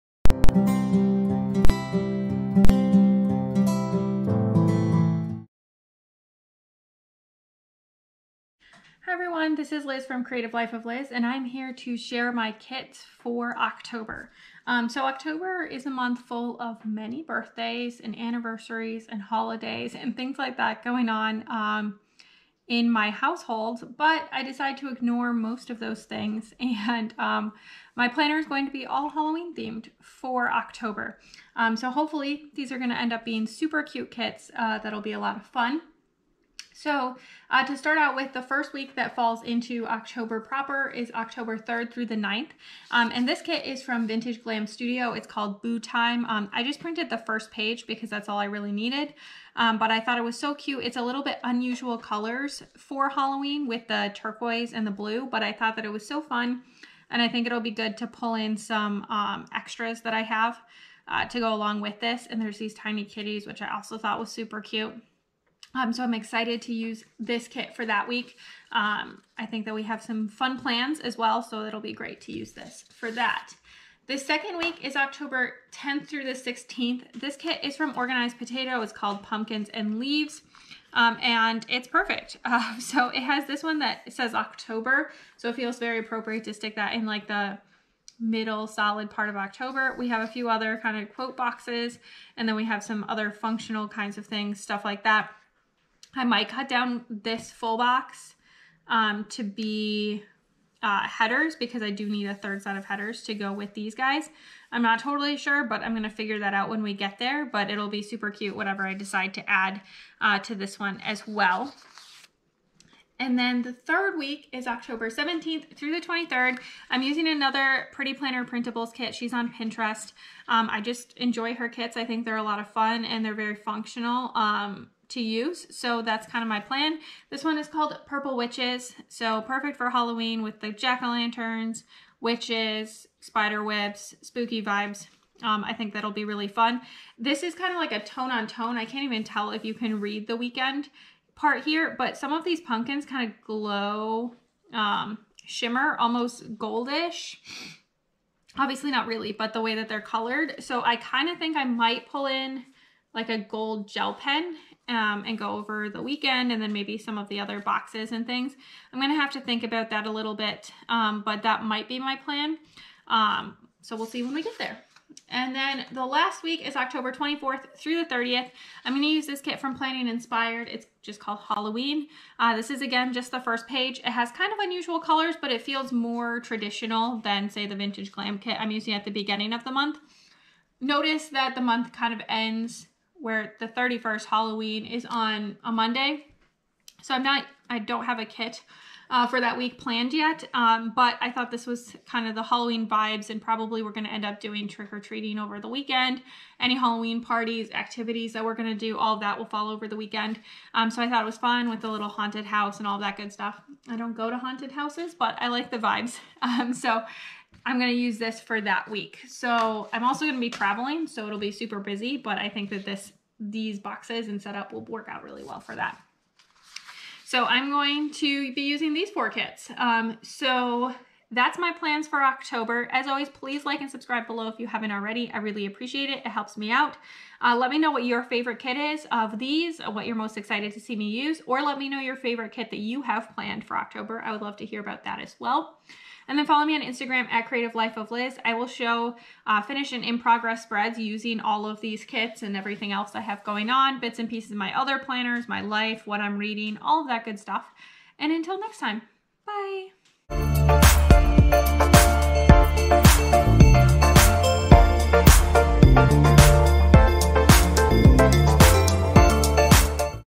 Kits I'm Using in October 2022 | Time for Halloween! | Recollections Planner
October may be a month of birthdays, holidays, and travel for me, but my planner is going to be full of Halloween! Let me know down below about your kit plans for October and what your favorite kit is from the ones I'm using!

The Prairie Planner

Check out your local Joann, Hobby Lobby, Michaels, Target and Dollar Tree for similar sets to the sticker books I used!

Tools:

Slice Tool (green version, but the same as mine)
Leftovers Sticker Book